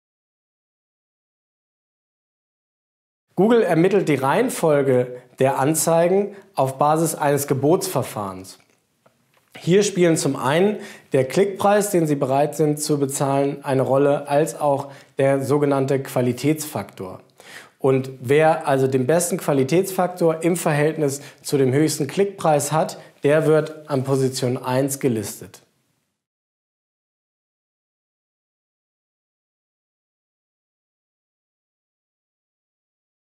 Suchmaschinenmarketing - GLOSSAR - Anzeigenauktionen
Dies ist ein Glossar-Video zur Reihe Suchmaschinenmarketing, in dem der Autor Prof. Dr. Eric Horster den Begriff „Anzeigenauktionen“ erklärt.

Original-Video:
Änderungen am Original: keine